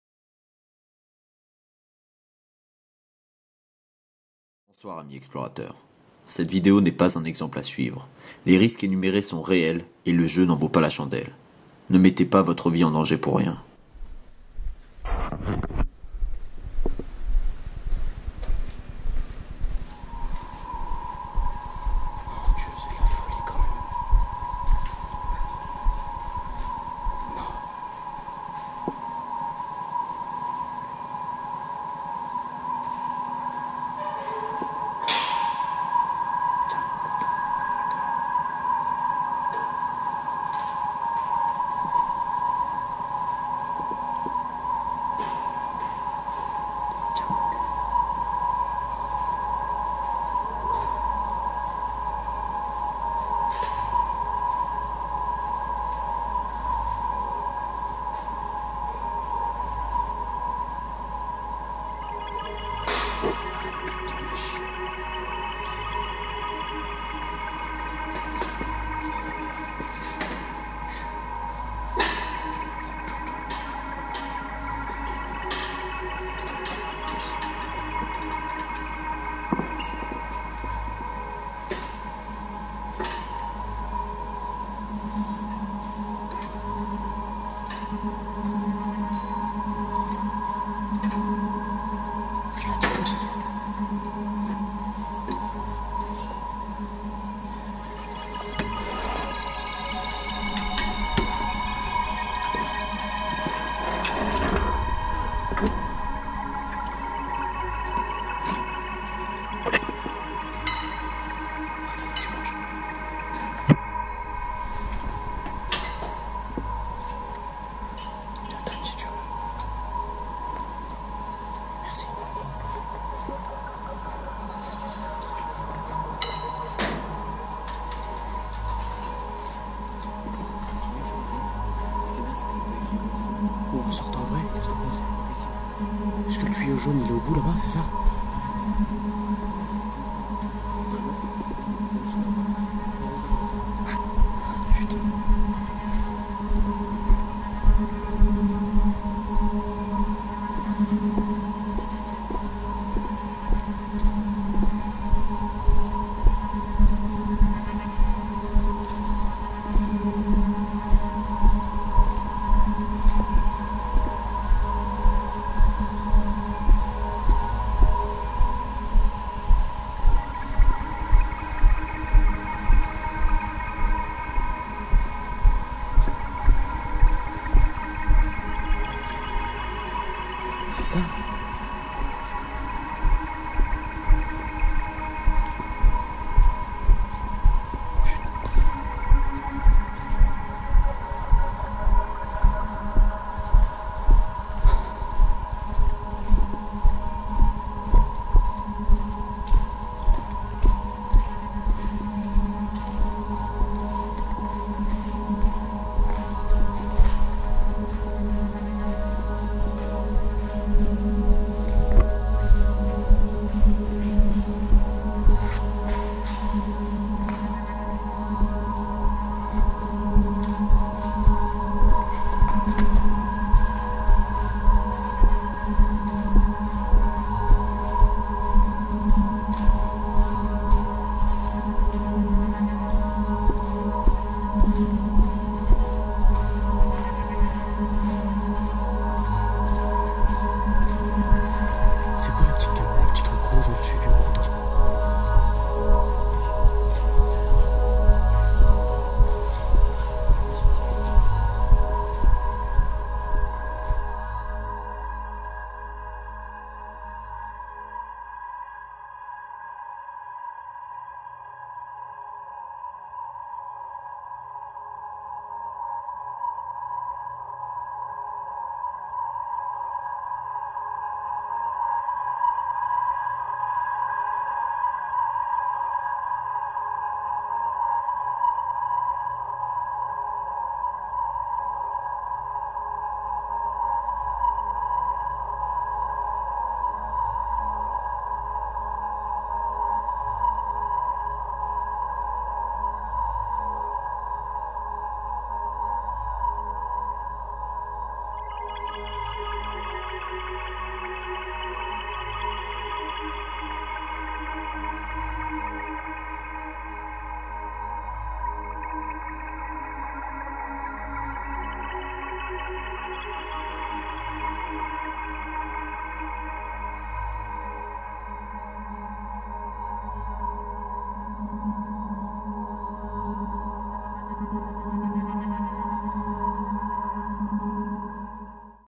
Tunnel du Grand Paris
Un tunnel pas comme les autres, les constructions hors normes des tunnels du grand Paris.